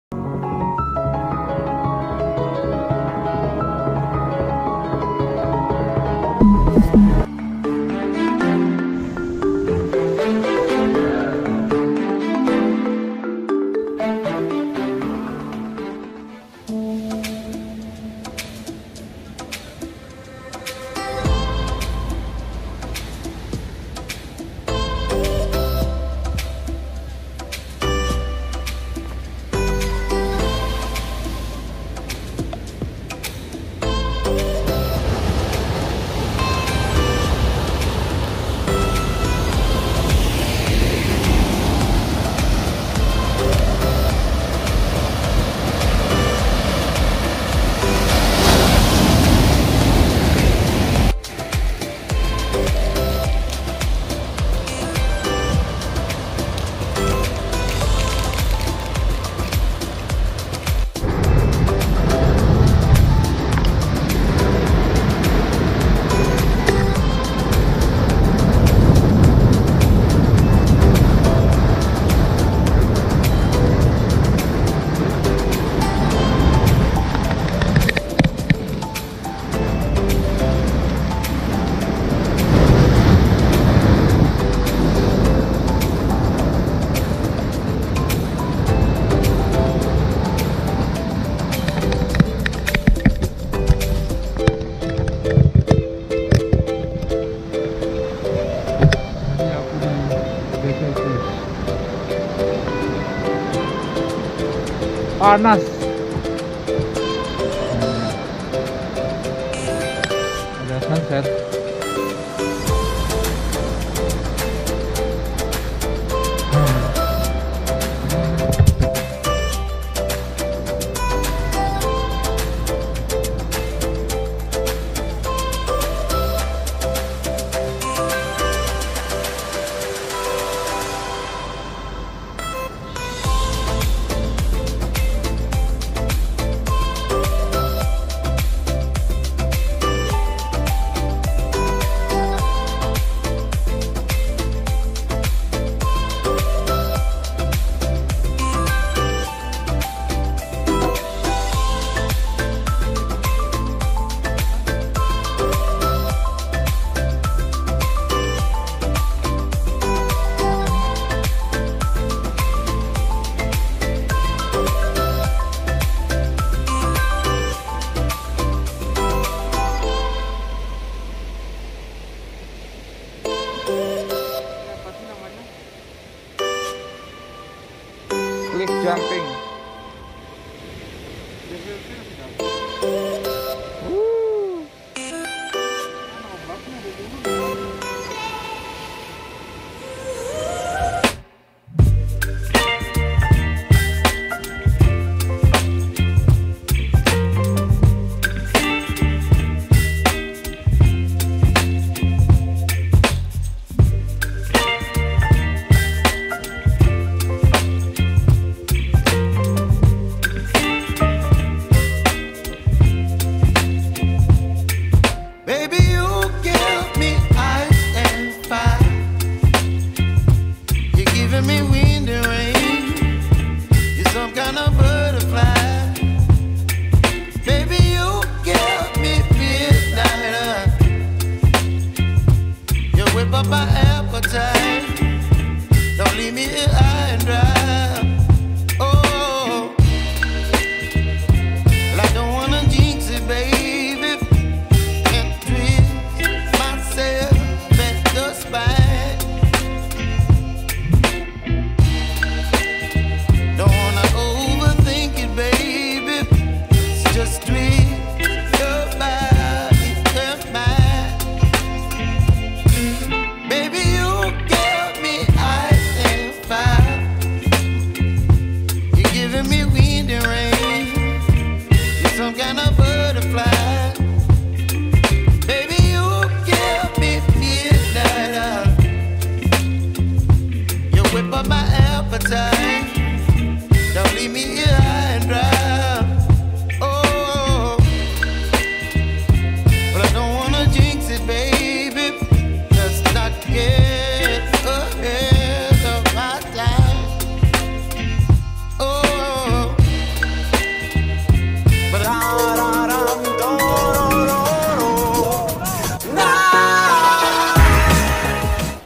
Blue Lagoon Cliff Jumping di Nusa Lembongan
Salah satu spot Cliff jump dan surfing di Nusa Lembongan